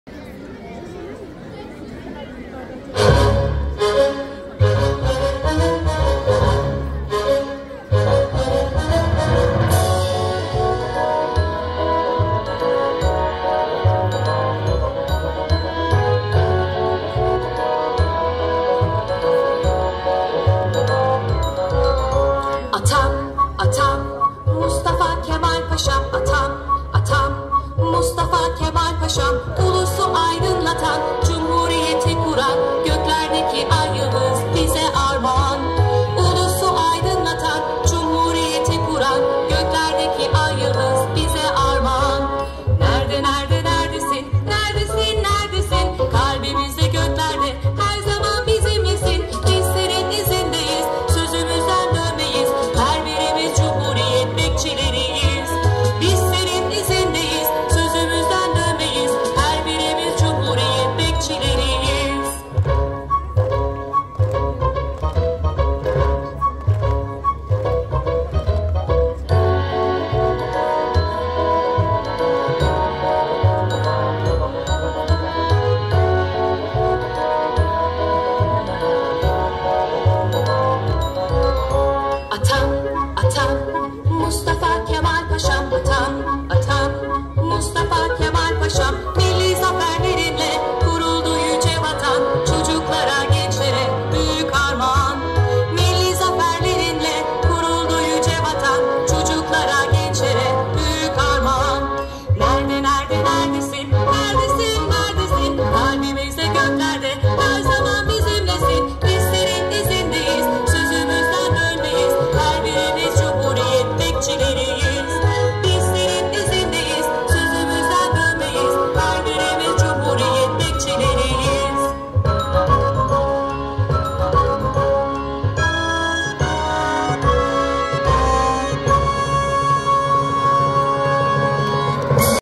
Atam
4 E sınıfı öğrencilerimizin 29 Ekim Cumhuriyet Bayramı gösterisi